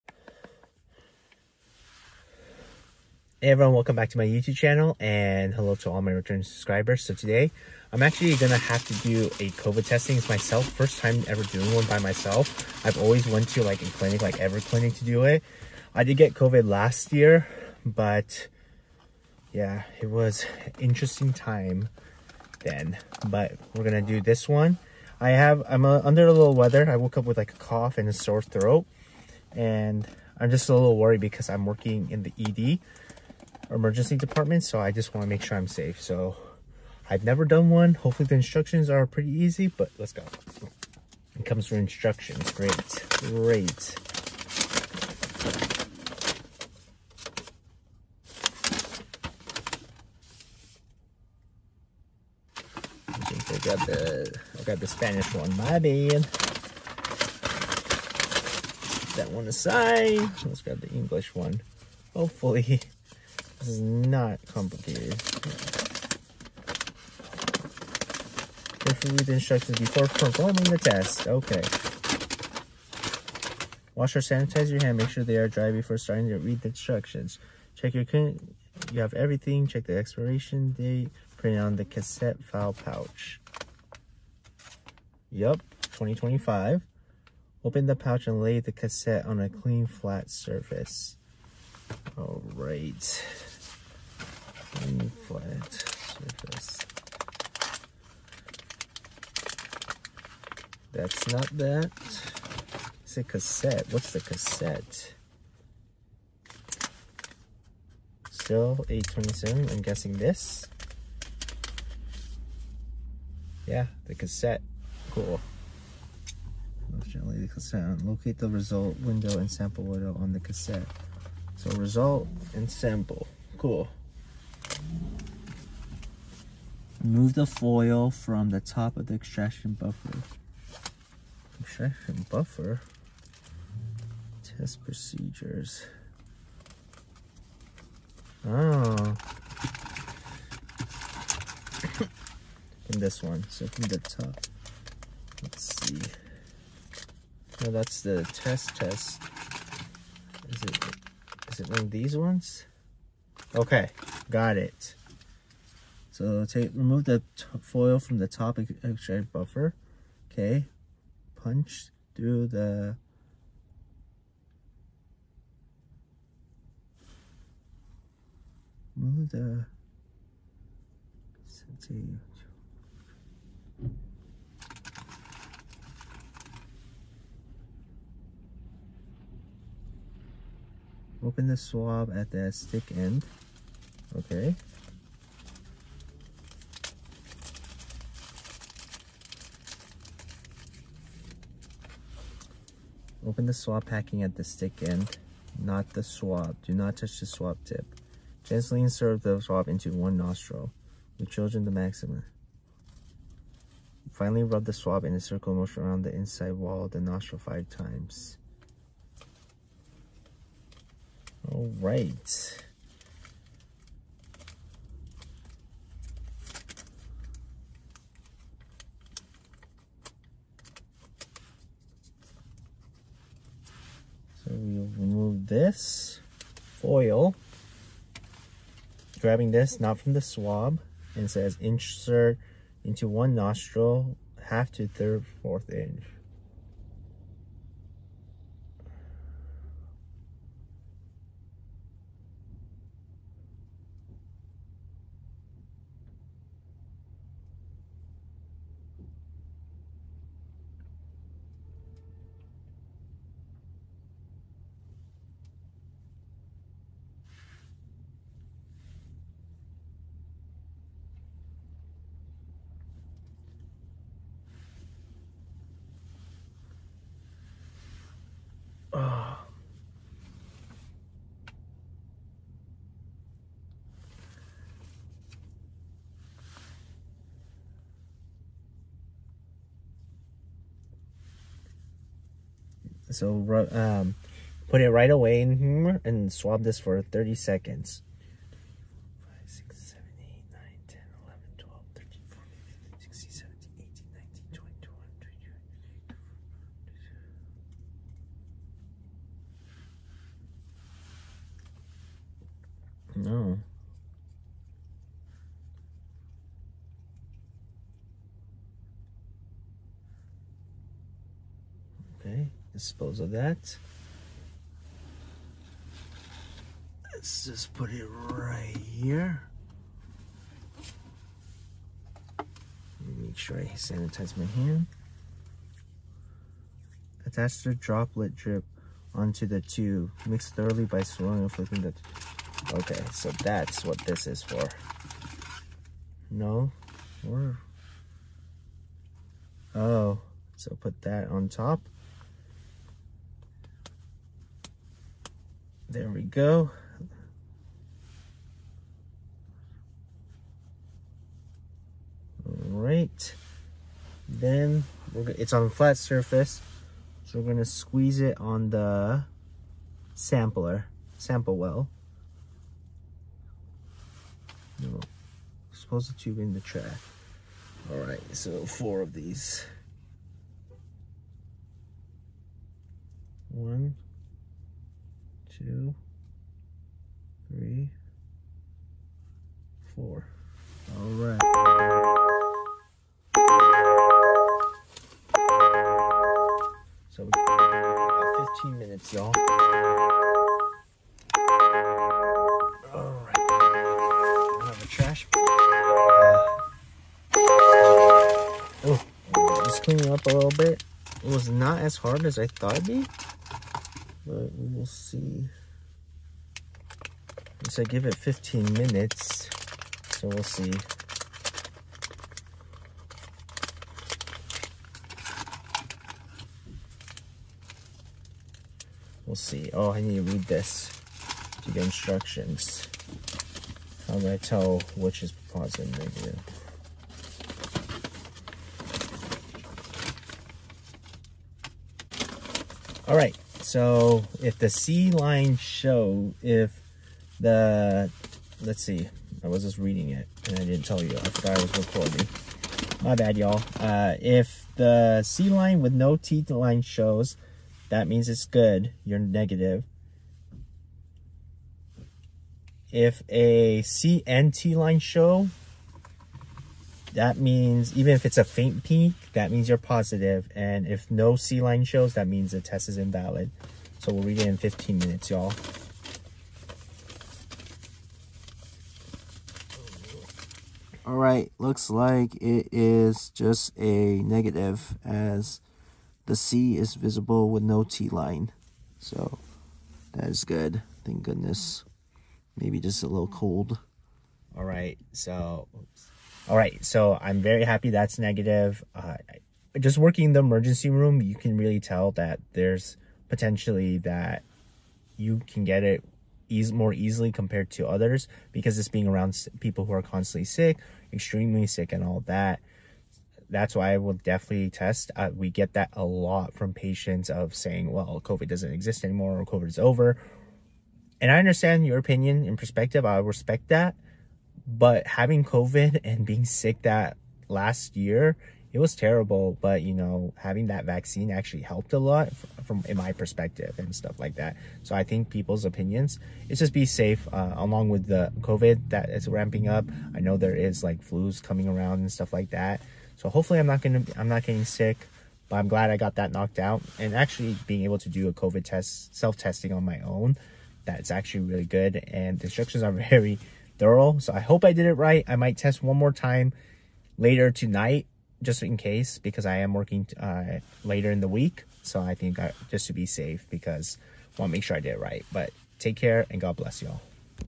Taking My First Self COVID Test!
I never did a self Covid test. I was feeling a little under the weather so I took one as I work in the Emergency Department and wanted to be safe. Here is how it went. Advice follow the instructions lol.